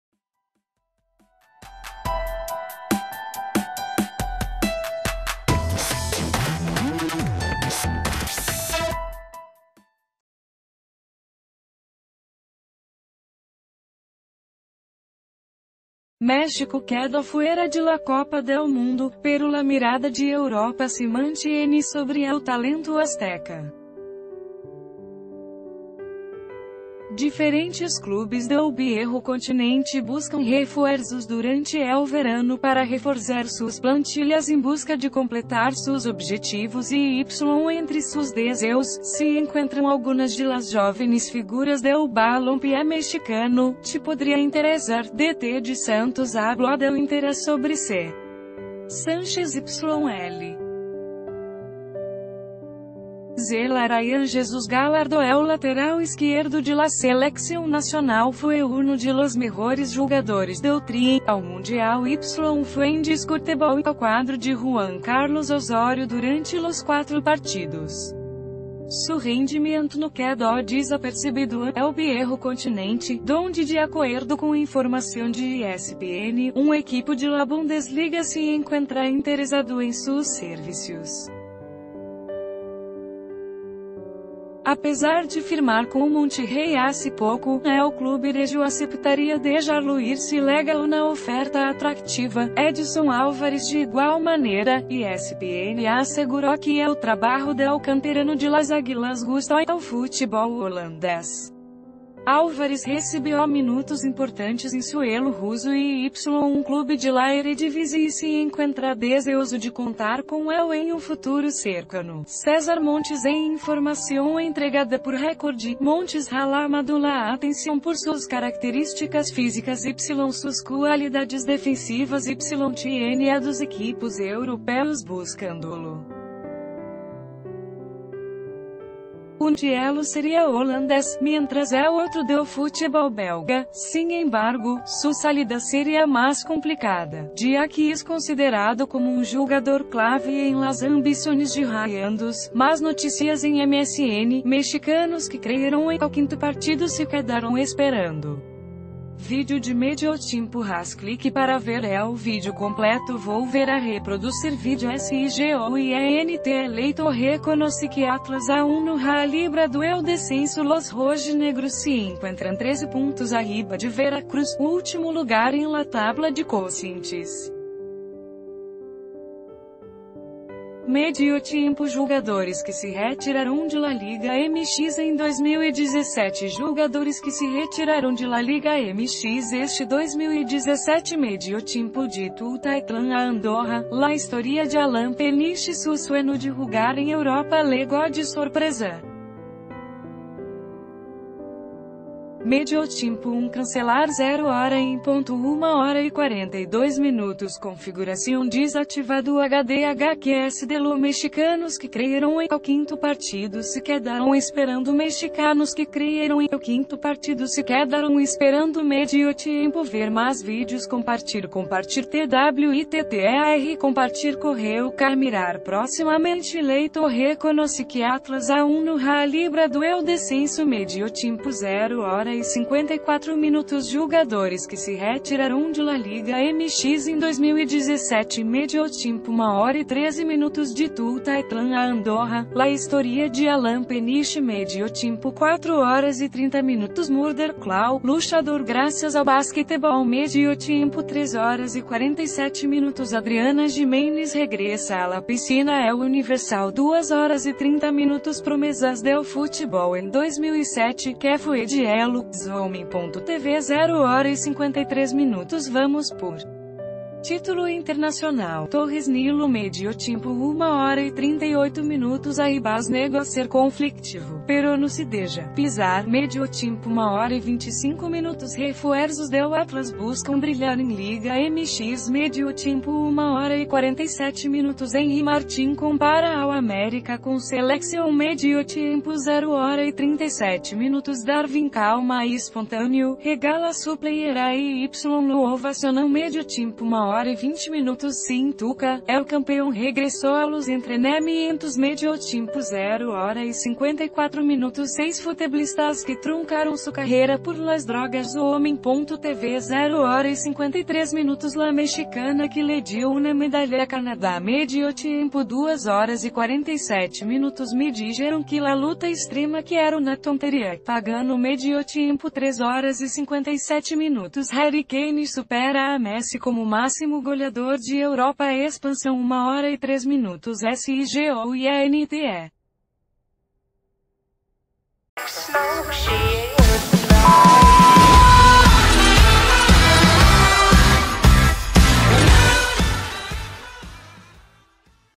En Europa ya buscan a jóvenes promesas de la Liga MX
México quedó fuera de la Copa del Mundo, pero la mirada de Europa se mantiene sobre el talento azteca. Diferentes clubes del Viejo Continente buscan refuerzos durante el verano para reforzar sus plantillas en busca de completar sus objetivos y entre sus deseos, se encuentran algunas de las jóvenes f...